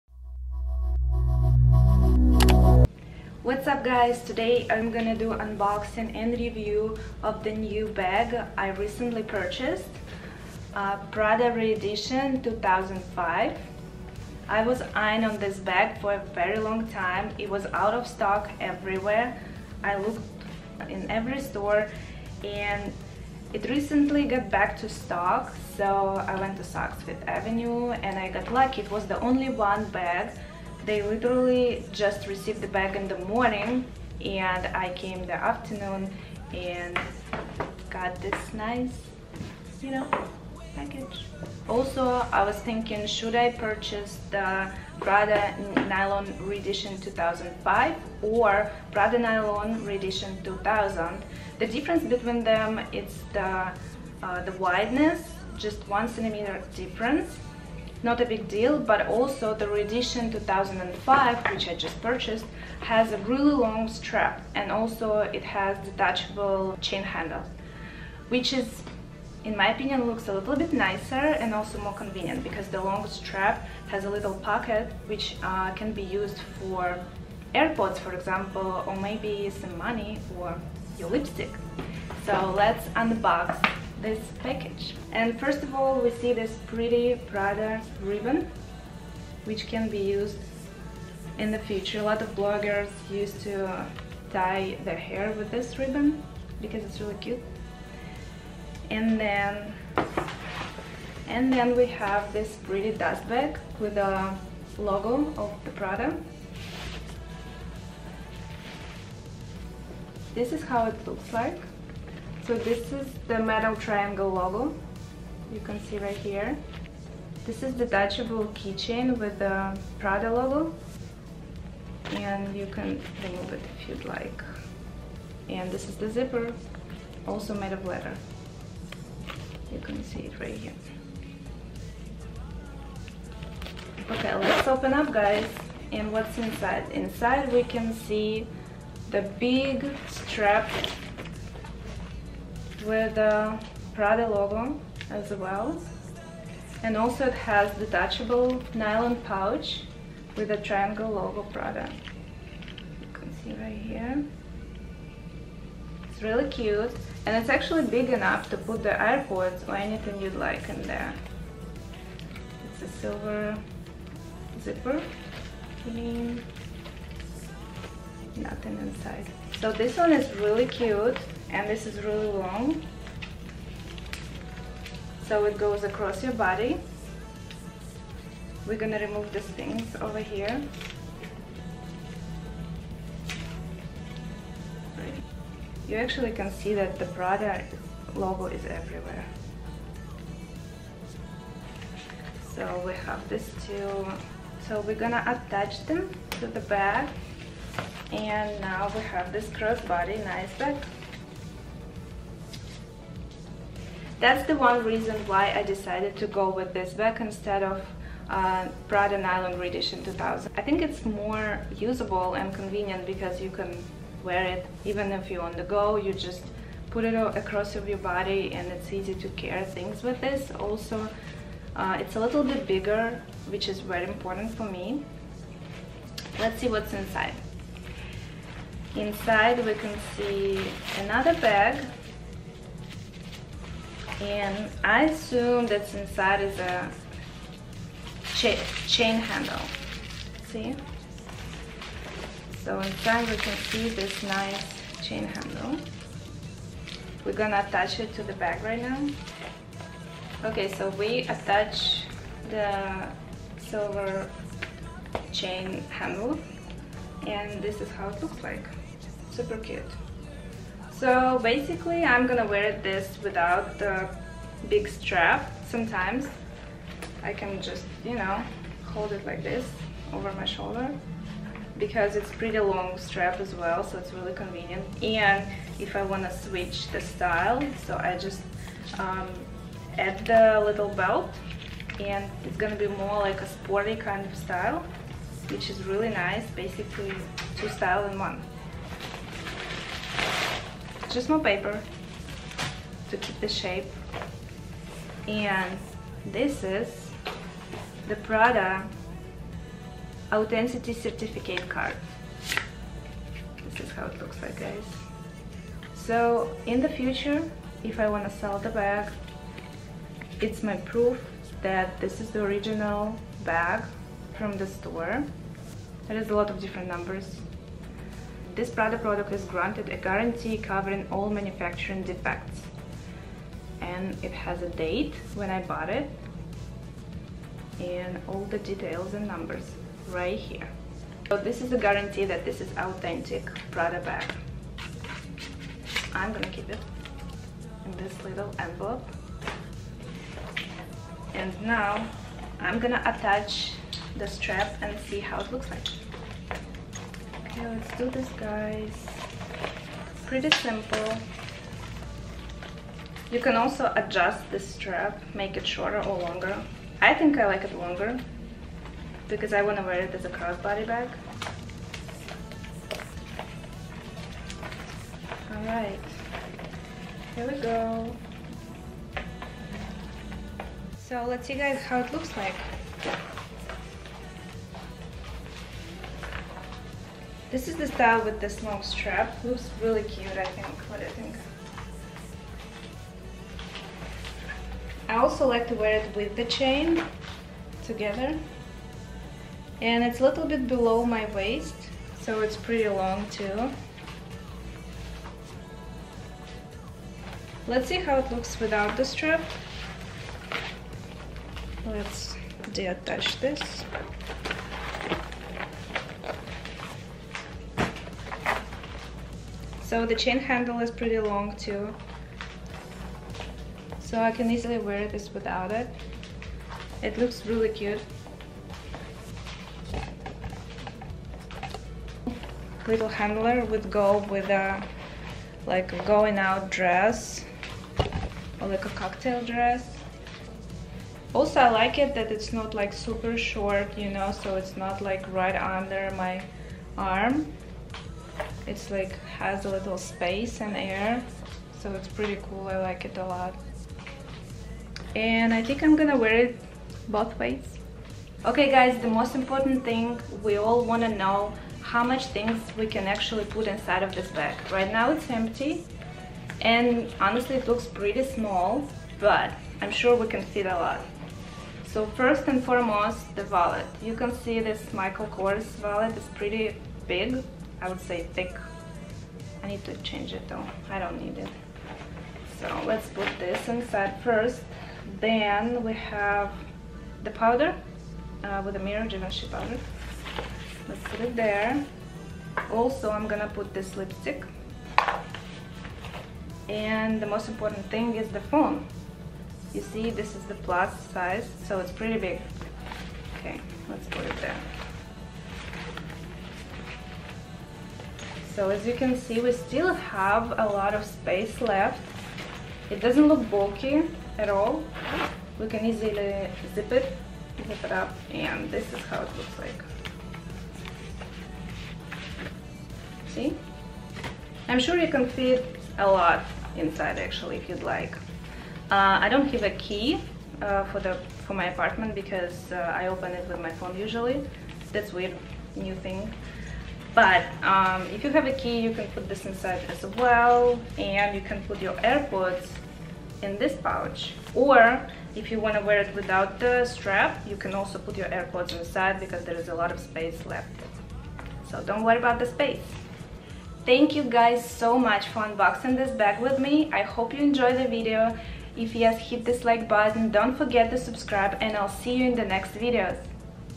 UNBOXING PRADA RE-EDITION 2005 NYLON SHOULDER BAG | REVIEW CROSSBODY BAG
In this video, I'm going to do Prada bag unboxing, and show you all the details, review the bag, and try to fit inside as much as possible